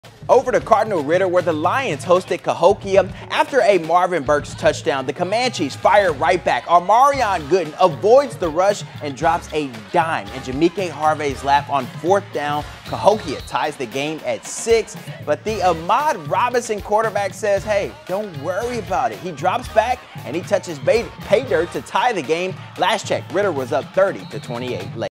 Cardinal Ritter pulls out late victory over Cahokia
The Lions got the Week 1 win over Cahokia.